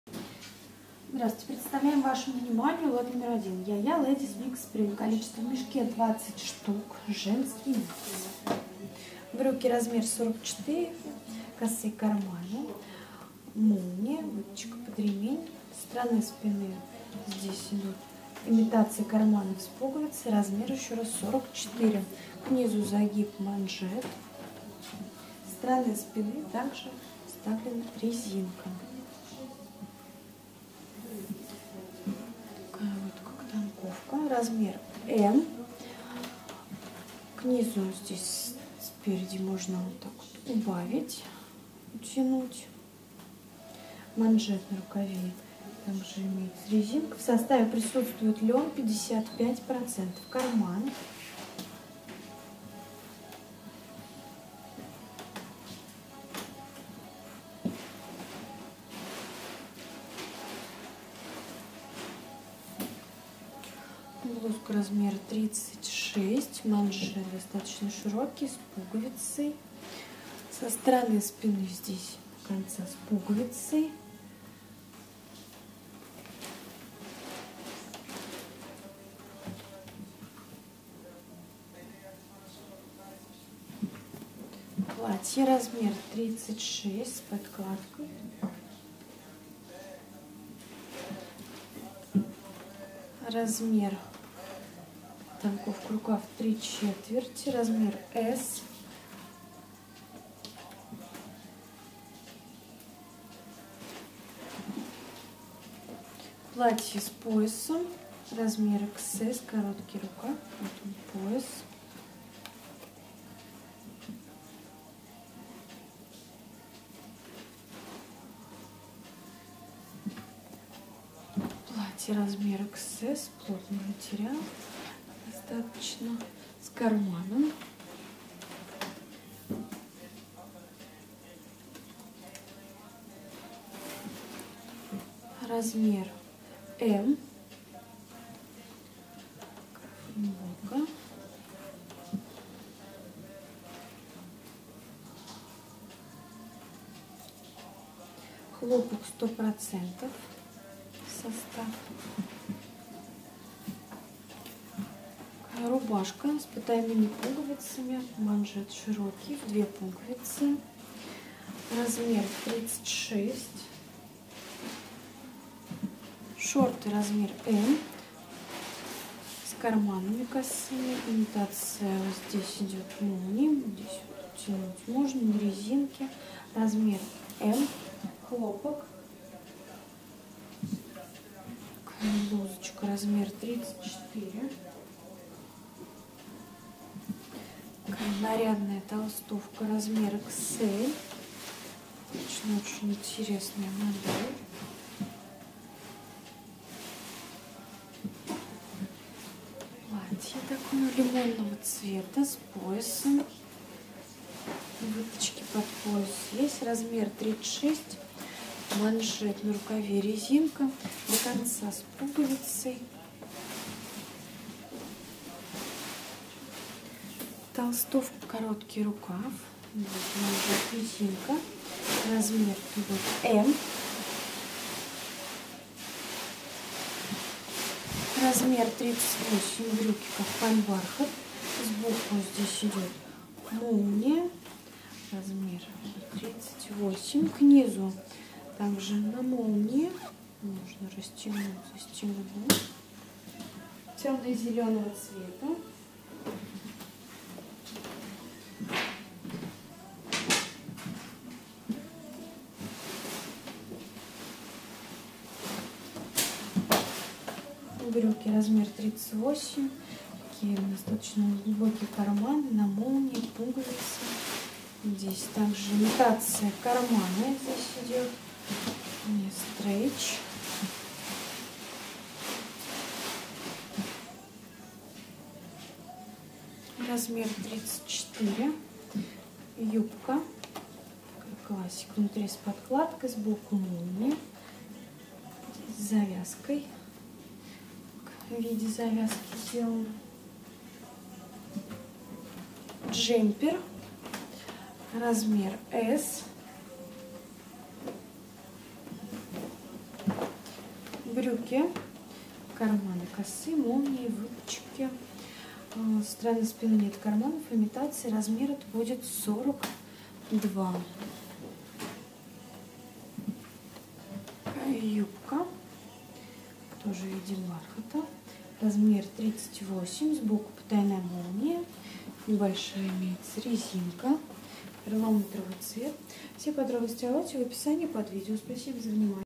YAYA LADIES MIX SPRING 1 ,Цена за ед : 9.93 евро, сток одежда оптом
YAYA LADIES MIX SPRING (лот № 1)
Женский микс (сток)
Страна: Голландия
Сезон: Весна
Цена за мешок: 198.60 евро
Кол-во в мешке ,шт.: 20
Цена за ед.: 9.93  евро